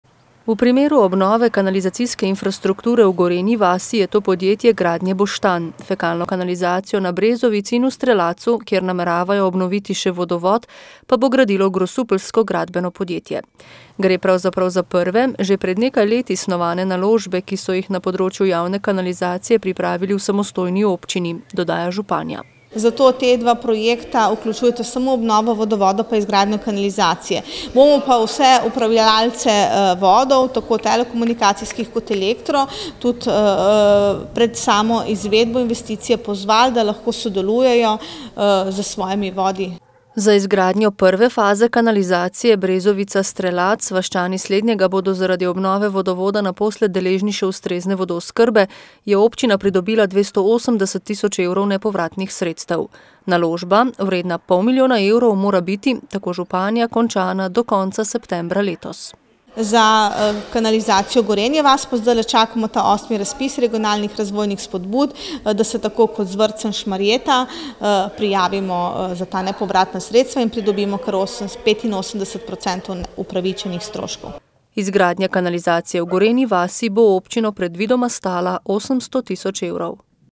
Obnova šole in gradnja kanalizacije
ŠMARJETA - Kar nekaj javnih naročil je v minulih tednih na svojem spletnem portalu objavila šmarješka občina. Medtem ko izbira izvajalca za energetsko sanacijo tamkajšnje osnovne šole še ni pravnomočna, pa sta že znana izvajalca del na kanalizacijskem omrežju v Gorenji vasi in Strelacu.